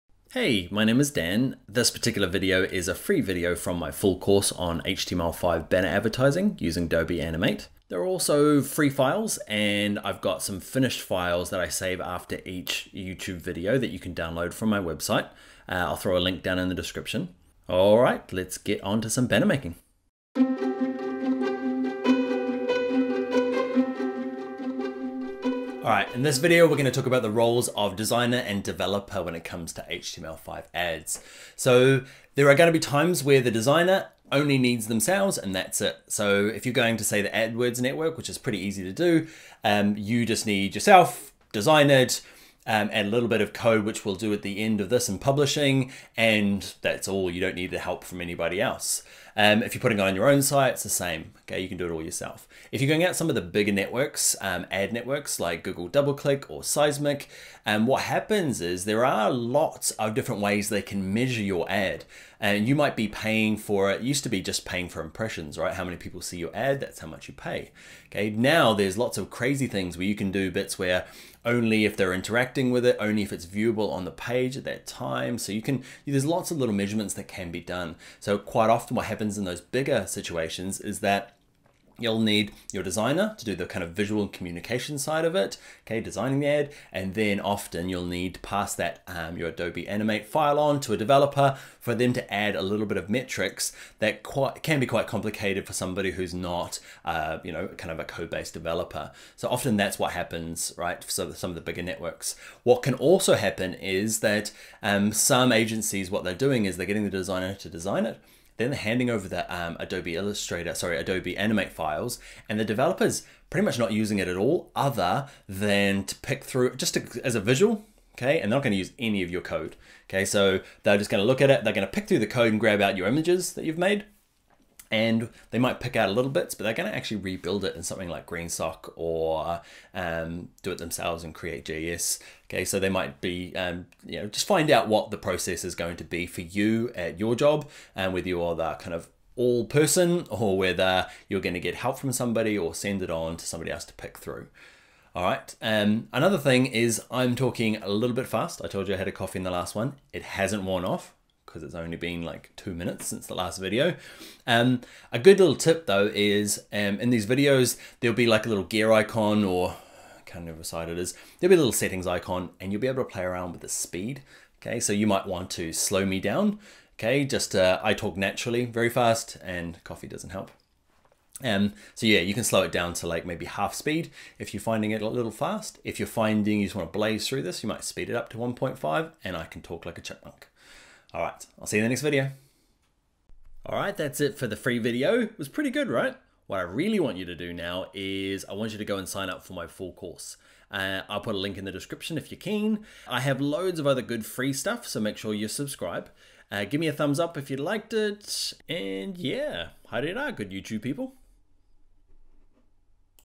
Roles - Designers vs Coders - HTML5 Banner Ads in Adobe Animate [8/53]
Join BYOL for just $1 for your first month, or $60 for 1 year this month only

Alright in this video were going to talk about the roles of designer and developer when it comes to HTML5 ads. So there are going to betimes where the designers only need themselves and that’s it. So if you're going to say the ad works network which is pretty easy to do you just need yourself, design it, add a little bit of code which well do at the end of this in publishing. And that’s all you don’t need the help from anybody else. If you're putting it on your own site it’s the same, you can do it all yourself.